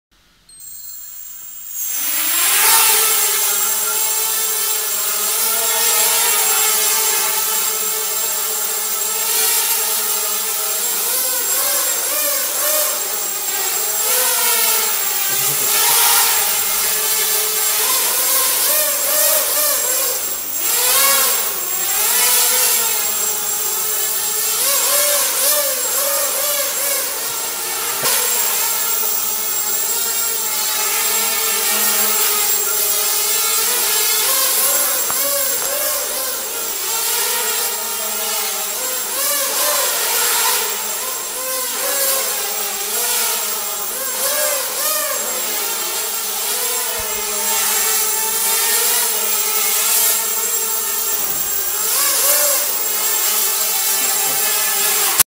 ZMR250 miniquad yaw wobble
My ZMR250 with Cobra MR12B ESCs and Cobra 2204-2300Kv motors has this weird wobble when changing yaw direction fast from one side to the other...

Curiously my other ZMR250 with LittleBee 20A ESCs had this issue  too at first n stock settings with CleanFlight...  but stoped doing it when i flashed it to BetaFlight and set the LittleBees to damped-light , oneshot and timming med-high.

This older ZMR with Cobra ESCs didnt liked that setup tought..so here is back to CleanFlight with stock PIDs , yaw rate 0,15 and looptime 2000.

Also im indoors in the garaje because winter